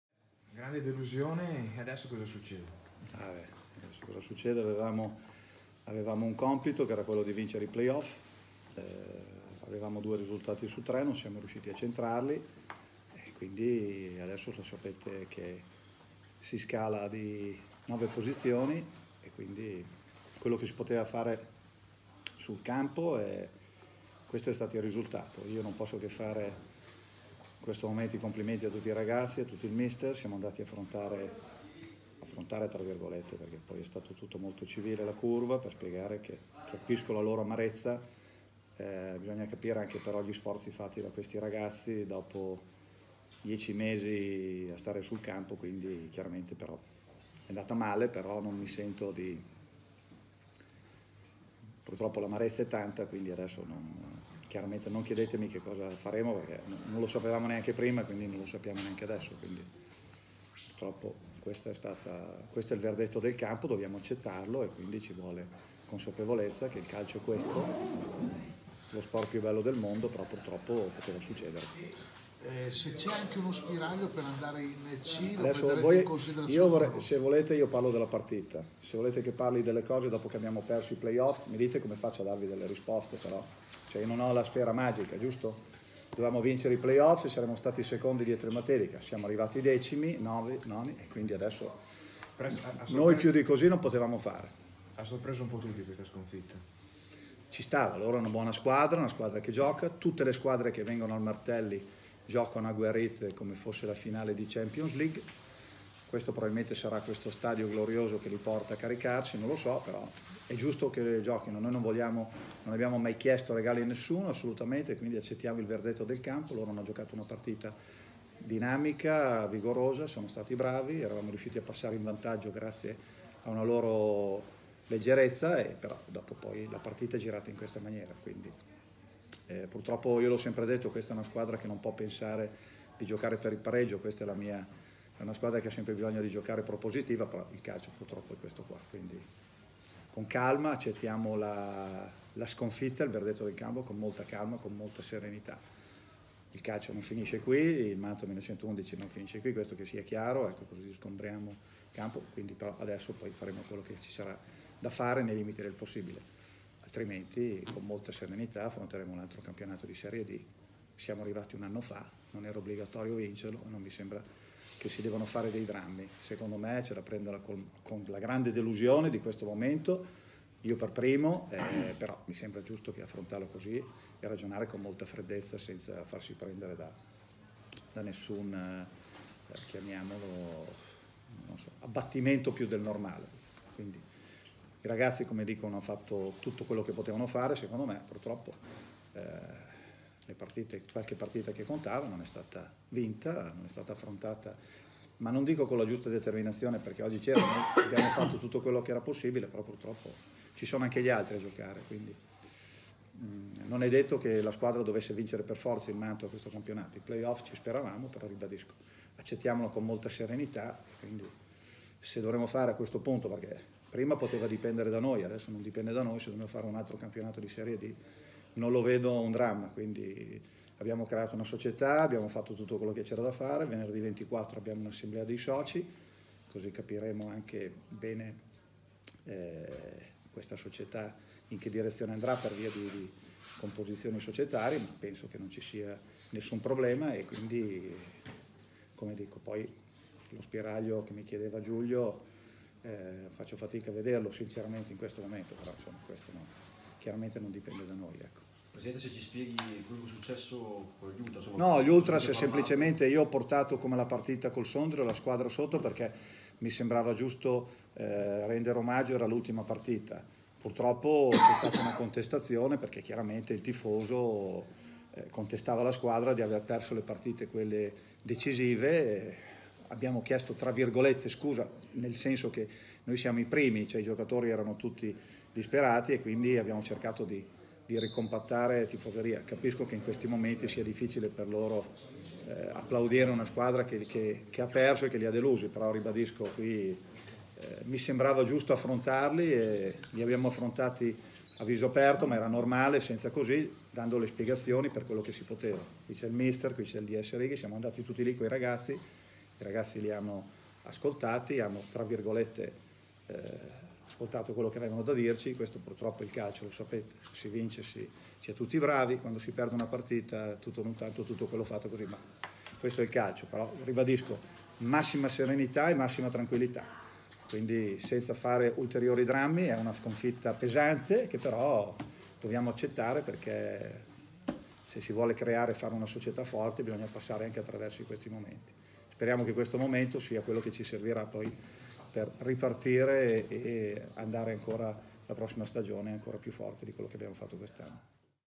Mantova, persi i playoff. Masiello: grande delusione, ma non facciamo drammi
Il presidente del Mantova amareggiato per la sconfitta col Pro Sesto alla finale dei playoff. "Comprendo i tifosi della curva, ci siamo confrontati con loro. Ma capisco anche i ragazzi della squadra, che hanno fatto un gran campionato. Sia chiaro che Mantova 1911 non finisce qui. Vorrà dire che ci prepareremo per un nuovo campionato di serie D".